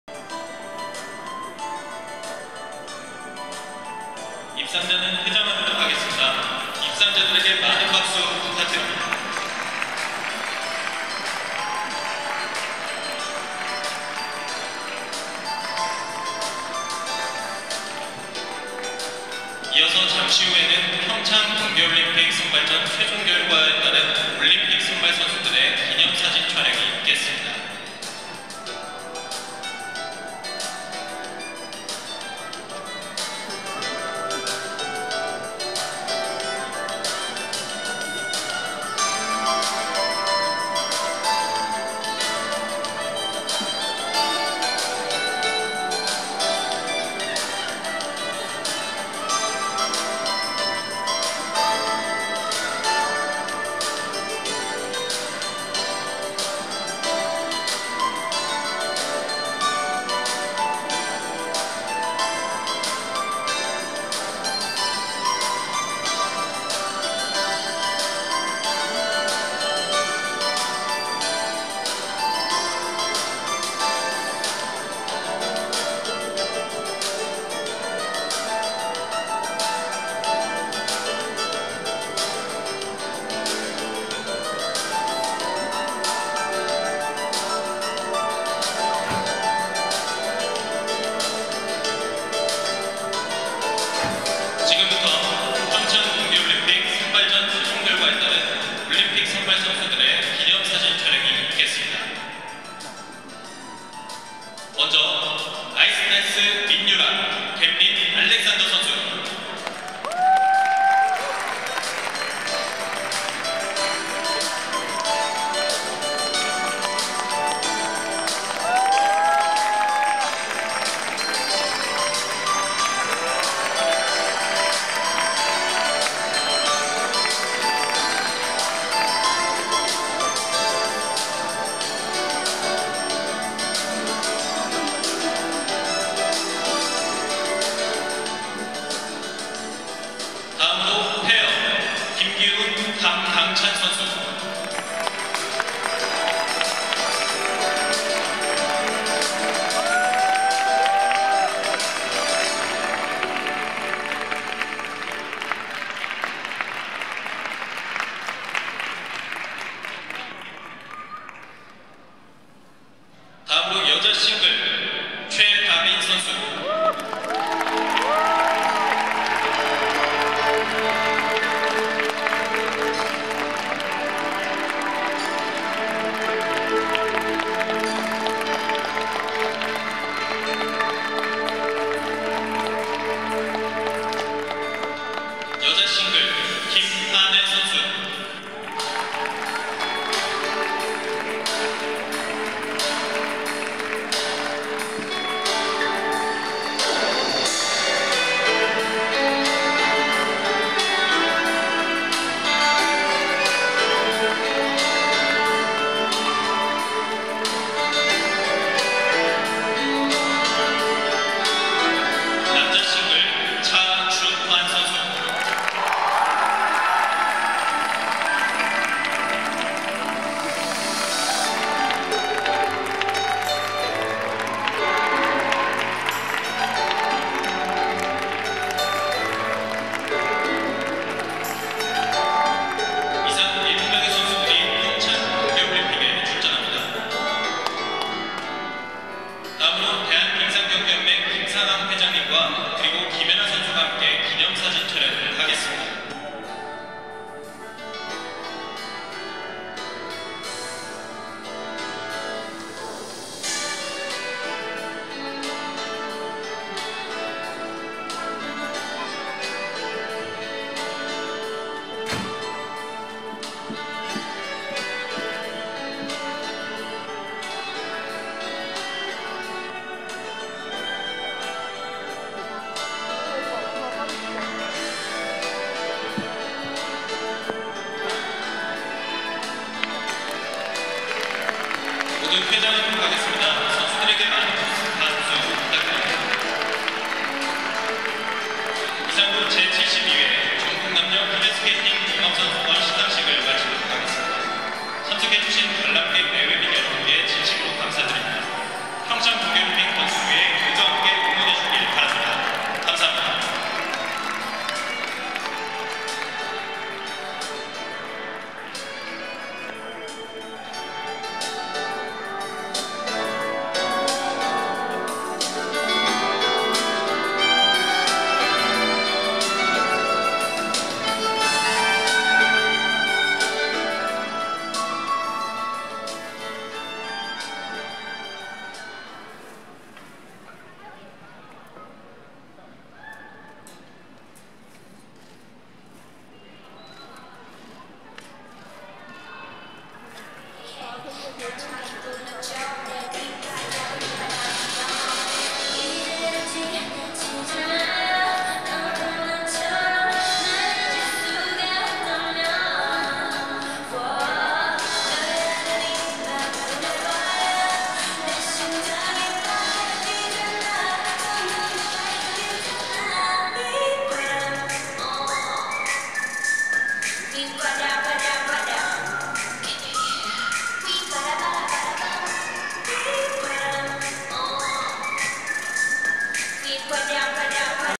2018-01-07 피겨종합선수권 3일차 | 올림픽 출전권 확정선수 기념사진 촬영 | 김연아 Yu-NA KIM, 최다빈,차준환,김규은&감강찬, 민유라&겜린
차준환 Jun-Hwan CHA,
김하늘 HaNul KIM,
President of KSU,
최다빈 Dabin CHOI,
김연아 Yu-Na KIM,
김규은  & 감강찬 Kyu-Eun KIM & Alex Kang-Chan KAM,
민유라 & 알렉산더 겜린 Yura MIN & Gamelin Alexander,

72th KB Bank, Republic of Korea National Championships 2018 (South Korea National Championships) | Day3 | (Jan.07) 2018 Mokdong Ice Rink, Seoul, Republic of Korea (South Korea) |
제72회 전국 남녀 피겨스케이팅 종합선수권대회 | 대회 3일차 | 서울 목동, 목동 아이스링크
Ice Dance Senior FD | Pair Senior FS | Group1 Men Single (=Senior Men) FS | Group1 Ladies Single (=Senior Ladies) FS | Victory Ceremony
아이스댄스 시니어 프리댄스 | 페어 시니어 프리스케이팅 | 여자시니어 (싱글1그룹) 프리스케이팅 | 남자주니어 (싱글1그룹) 프리스케이팅 | 시상식 |